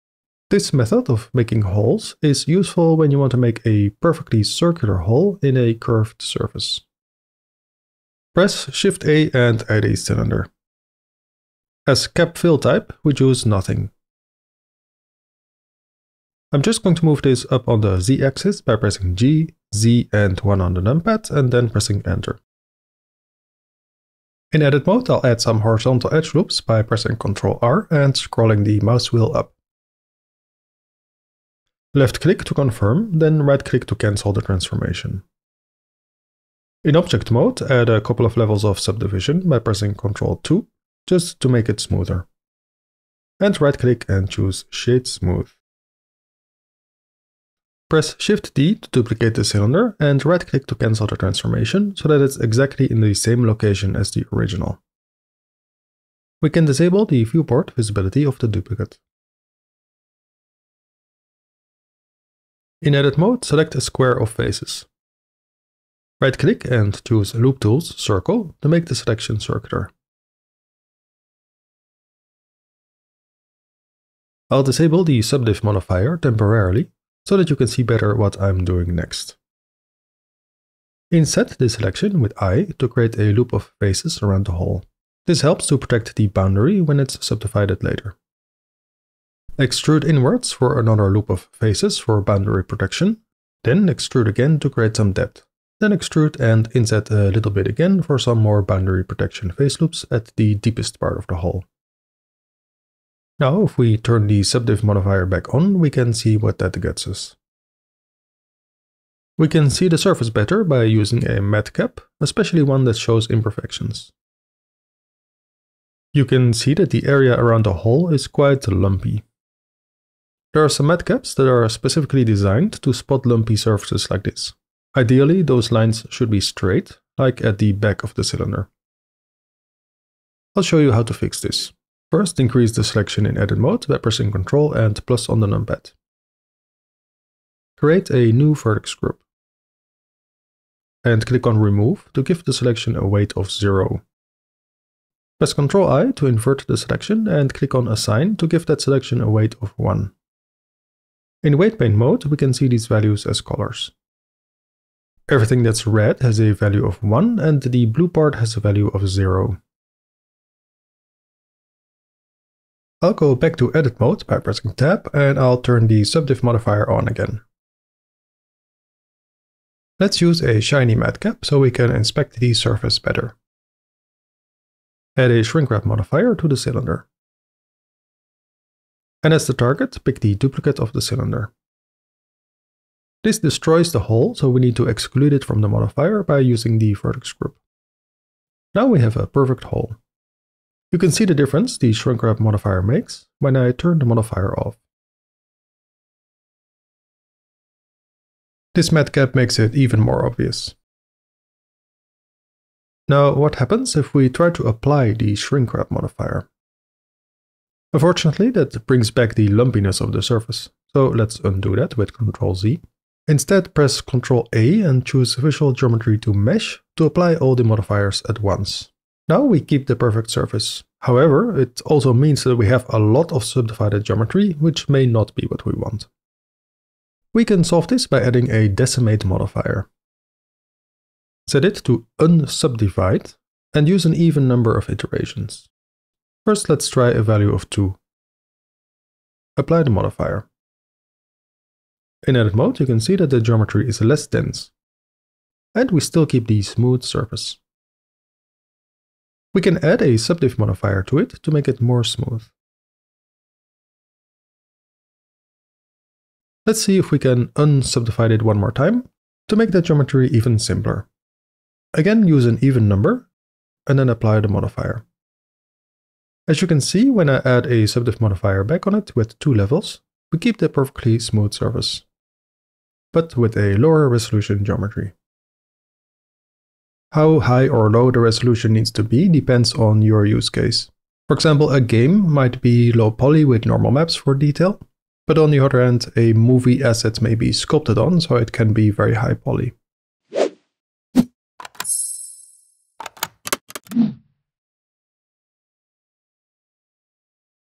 Perfect Holes with Quad Topology in Curved Surfaces - Step by step Blender beginner version
This technique is great for when you want a perfectly circular hole in a warped or curved surface. This is a step by step Blender beginner version of this topic.